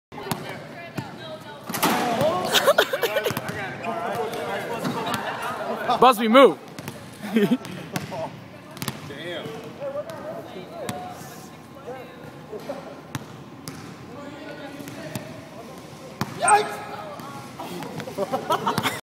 Big Sexy getting Dunked on by FGCU Chase Fieler
Florida Gulf Coast aka Dunk City's Chase Fieler dunking on the infamous Big Sexy